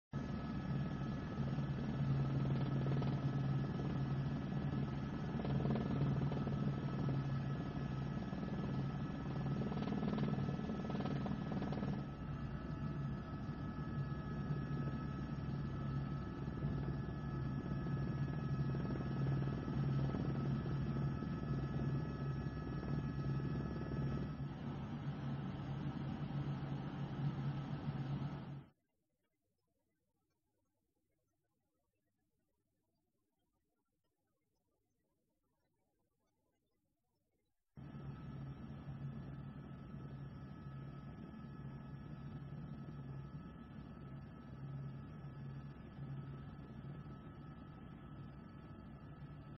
Thác nước chảy ngược ở Australia - Waterfalls flow backwards in Australia
Gió mạnh tại miền nam Australia khiến dòng nước chảy xuống từ ngọn thác phía nam Sydney bị thổi bay ngược lên không trung.

BBC cho biết, lượng mưa đổ xuống Sydney của ngày qua bằng lượng mưa của cả tháng, vận tốc gió đạt 120km/h. Hiện, Australia đã ban hành mức cảnh báo sóng to có thể lên cao đến 5m.